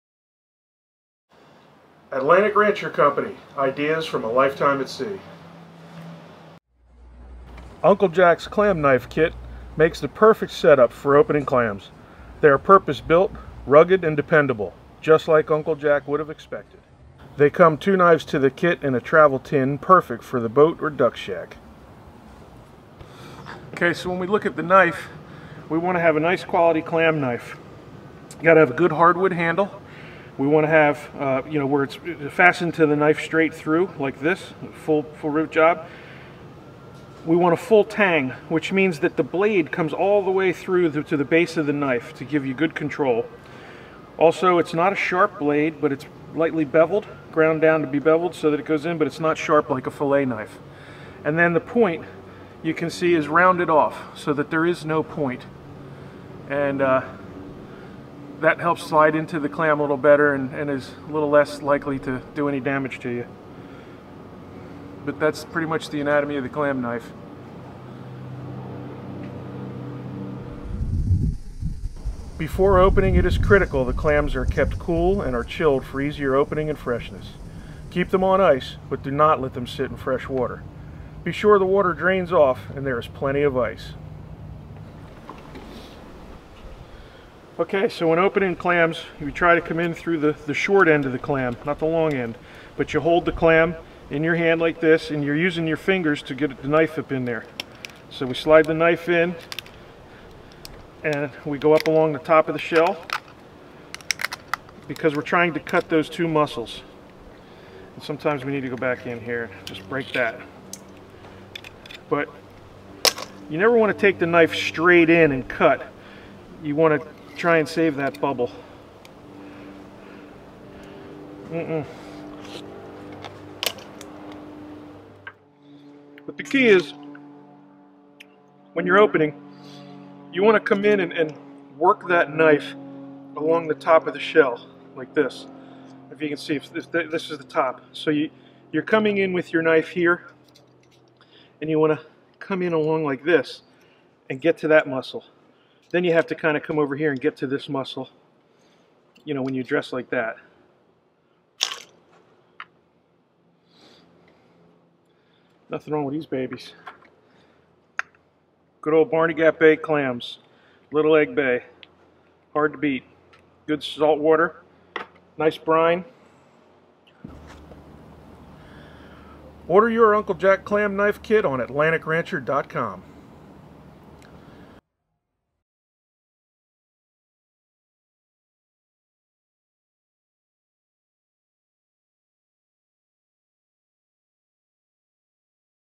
How to Open Clams the right way (The Best Clam Knife)
Having the right tools to do any job saves time and makes the job easier. That surely holds true for opening clams and oysters.

The Atlantic Rancher Uncle Jack clam knife is purpose-made not only to do the job but last a long time doing it, just like Uncle Jack would have expected. A good oyster knife and a quality clam knife with a properly shaped stainless steel rounded tip beveled semi-dull edge blade and a wooden handle with a full-length tang to fasten to for strength and hand comfort when your opening a few hundred at a time. So this kit is made to help you open clams right. Now we’re going to show you how to do it right with the best clam knife.

Support your local clammer, buy a hundred, get your knife, and have a party! That’s how we do it down by the bay...